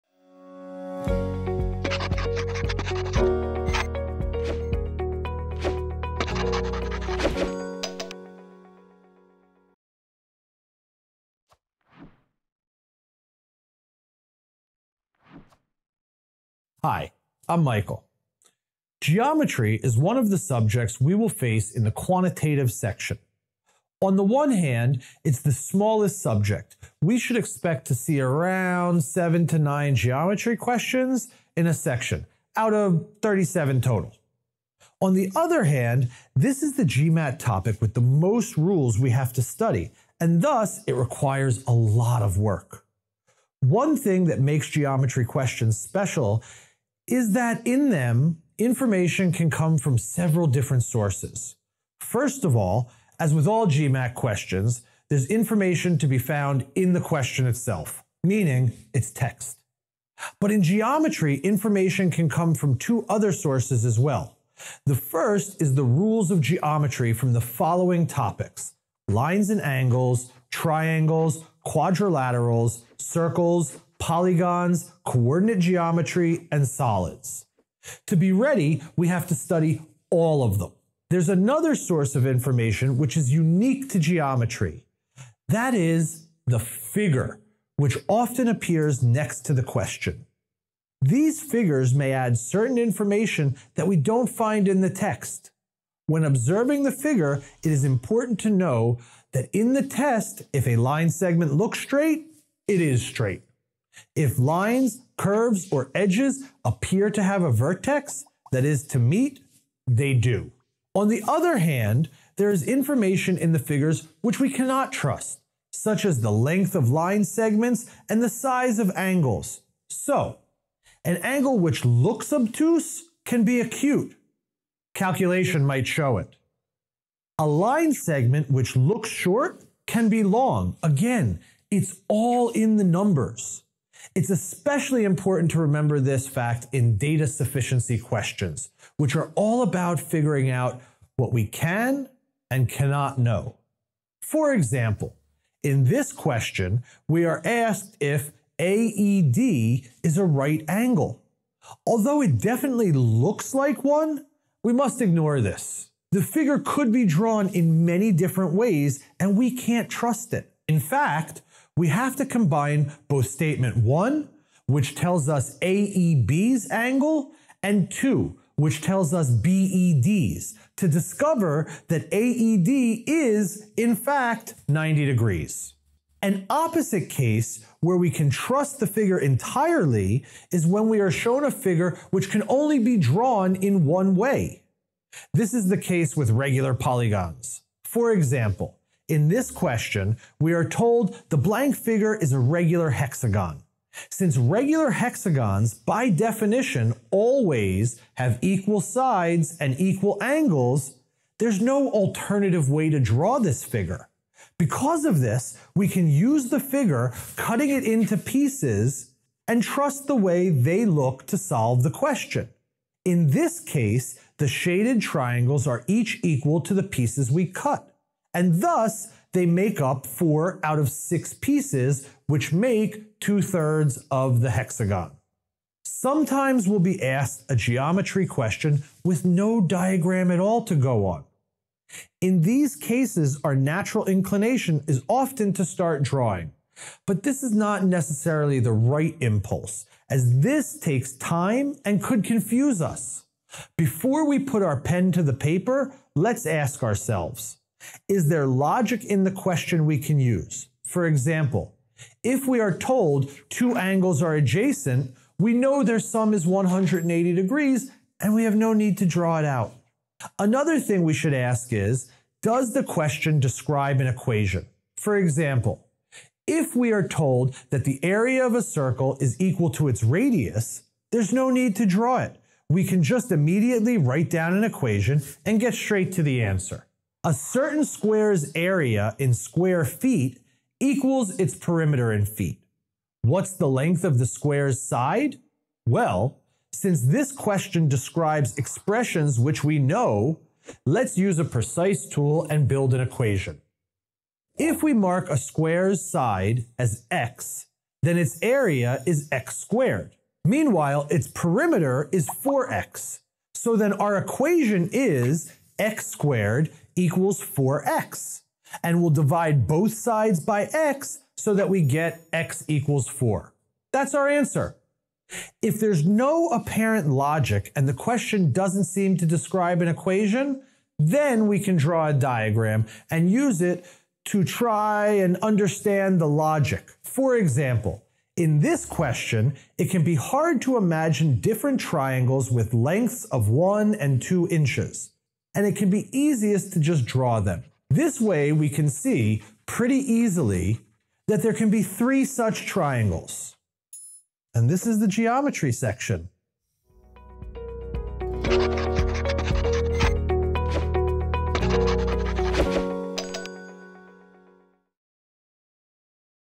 GMAT Tutorials: GMAT Quant – Geometry
The new GMAT tutorial series produced by PrepAdviser and examPAL discusses essential aspects of the exam and offers valuable preparation tips. This video explains more about mastering geometry in the Quantitative Section of the GMAT. Although geometry is the the subject given the least space in the exam, it requires a lot of work. In any given geometry question, the information included in the text can be somewhat tricky to interpret. Find out how to deal with lines, angles, and figures with confidence!